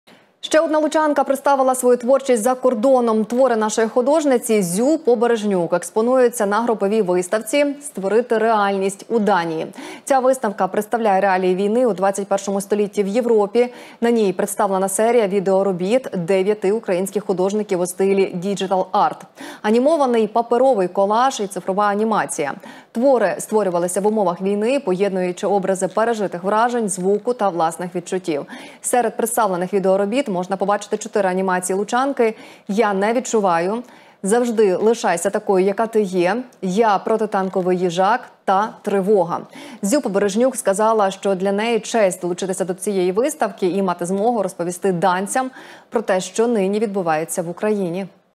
Лучанка представила свою творчість за кордоном
Лучанка представила свою творчість за кордоном. Твори нашої художниці Зю Побережнюк експонуються на груповій виставці «Створити Реальність» у Данії. Ця виставка представляє реалії війни у ХХІ столітті в Європі.